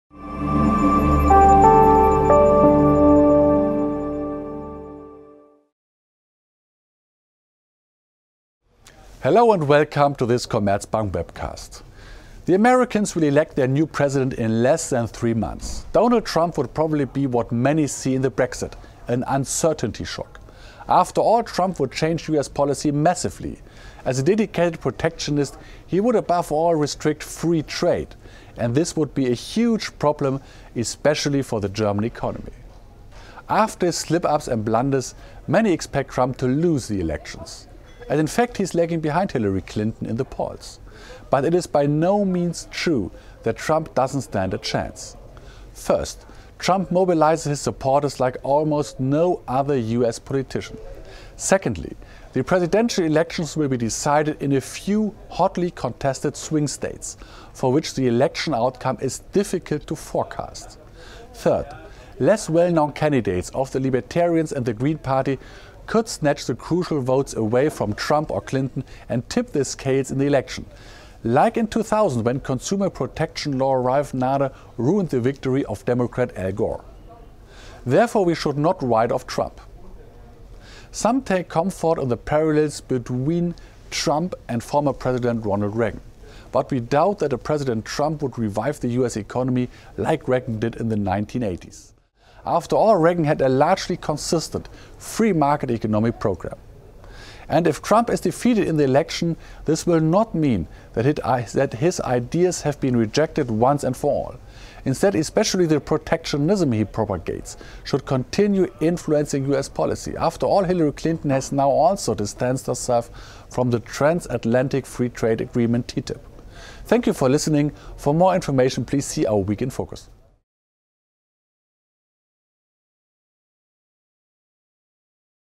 USA: Will it be Trump?
12 August 2016: According to the latest polls Donald Trump’s approval rating has fallen behind that of Hillary Clinton. But analysis of the US electoral system indicates that Mr Trump does stand a chance. Indeed, the presidential election will be decided in some fiercely contested states where the outcomes cannot be predicted with certainty.

For more information please click refer to the web site of our Chief Economist